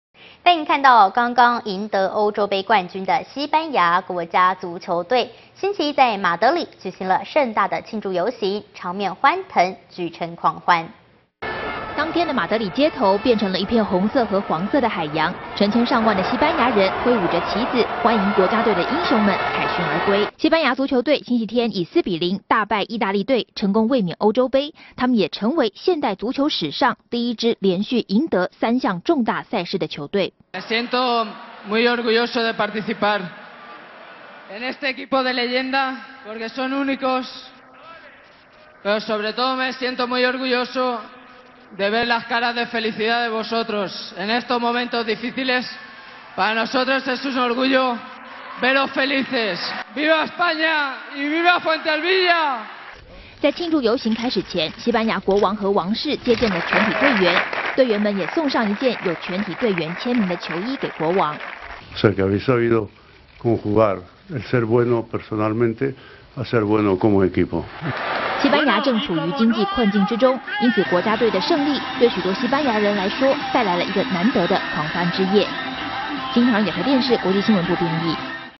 西班牙奪冠 馬德里遊行慶祝
【新唐人亞太台2012年7月4日訊】奪得歐洲盃冠軍的西班牙國家足球隊星期一在馬德里舉行了盛大的慶祝遊行，場面歡騰，舉城狂歡。

當天的馬德里街頭，變成了一片紅色和黃色的海洋。成千上萬的西班牙人揮舞著旗子，
歡迎國家隊的英雄們凱旋而歸。

西班牙足球隊星期天(7月1日)以4-0大敗義大利隊，成功衛冕歐洲盃，他們也成為現代足球史上第一支連續贏得三項重大賽事的球隊。

西班牙隊中場 伊涅斯塔：「我很榮幸成為這個傳奇球隊的一員，因為他們是獨一無二的。我特別自豪的是，能夠在這個困難時期看到你們開心的笑容，你們的歡樂讓我們感到自豪。西班牙萬歲﹗」

在慶祝遊行開始之前，西班牙國王和王室接見了全體隊員，隊員們也送上一件有全體隊員簽名的球衣給國王。

西班牙國王 卡洛斯一世：「你們表現的非常出色。你們將個人能力的發揮與團隊精神融為了一體。」

西班牙正處於經濟困境之中，因此國家隊的勝利對許多西班牙人來說，帶來了一個難得的狂歡之夜。

新唐人亞台電視 國際新聞部編譯

其他推薦:
看動態網：曉天下事